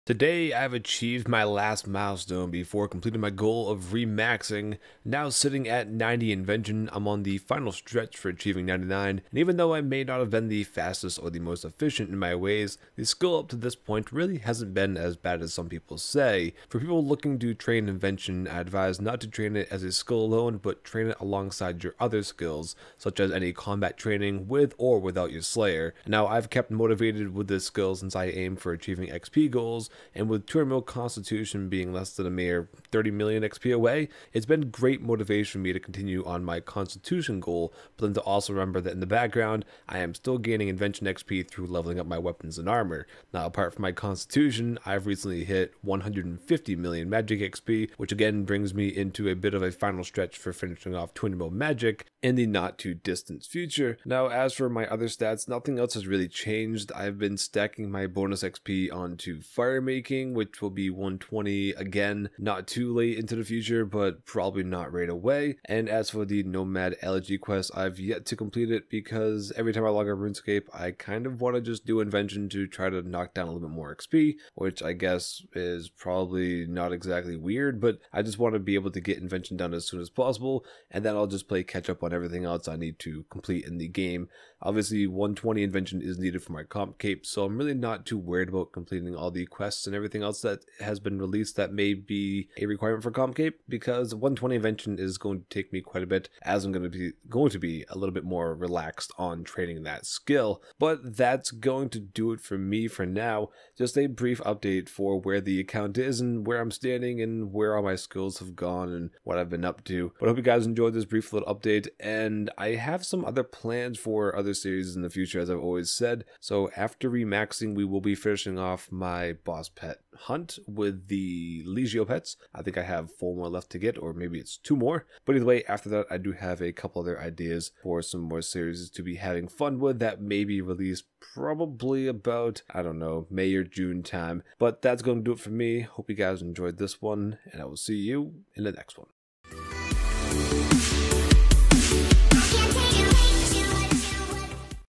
Account Update: Project Re Max Episode 2
Account update and getting 90 all skills. Explaining whats after the re max and where we stand!